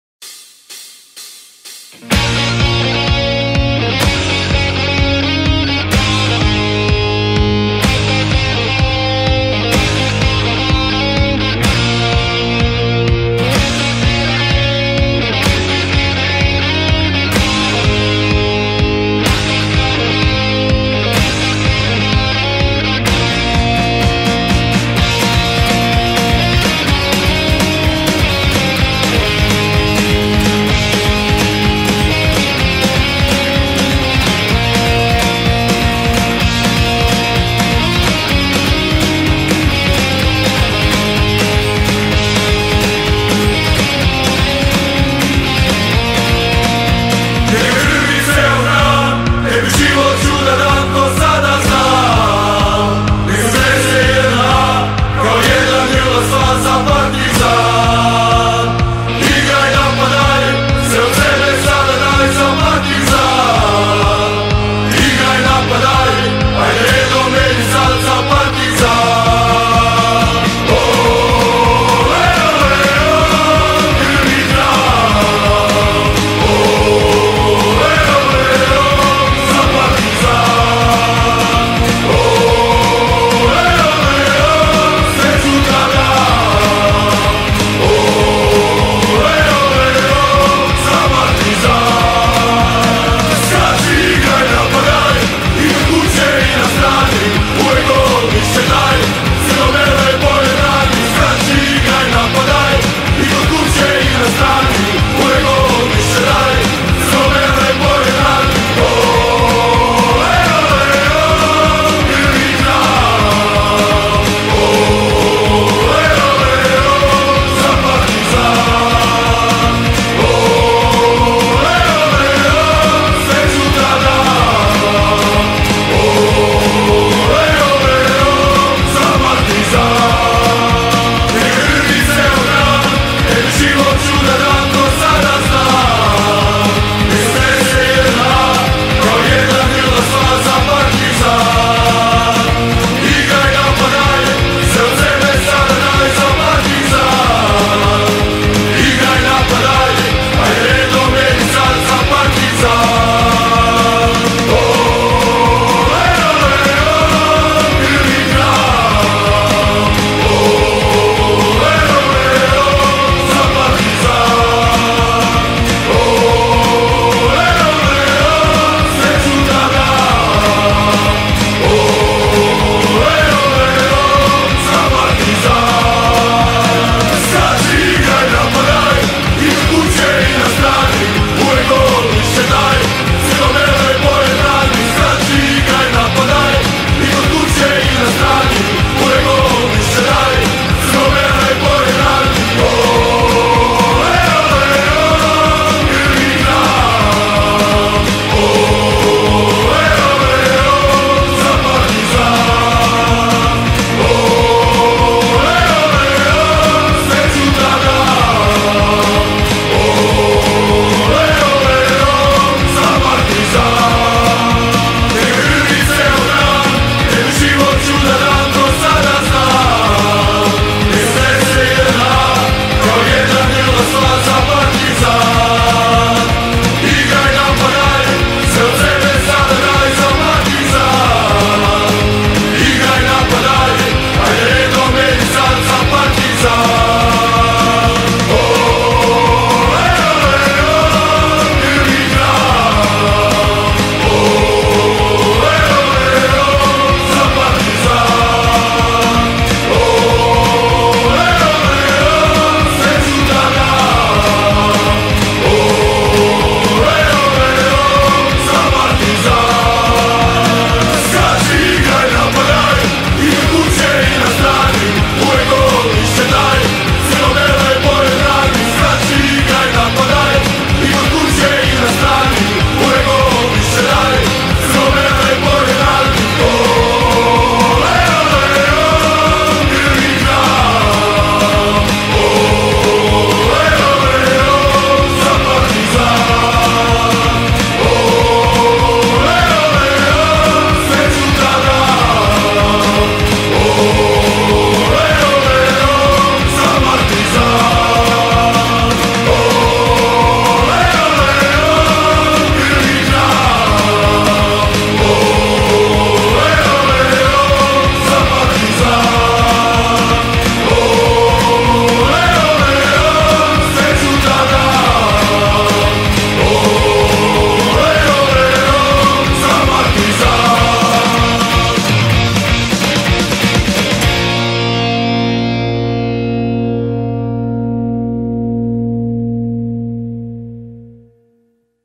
Strength and Conditioning Program for Youth Football Athletes I FK Partizan Belgrade
Neuromuscular training improves both physiological and physical measures associated with high-level performance.

It seems that strength and power training programs should target all the force-velocity potential/spectrum of the neuromuscular system.

Due to the conditioned concurrent nature of the sport, combined strength and combined high-intensity training approaches may constitute a good training approach within a football periodized process.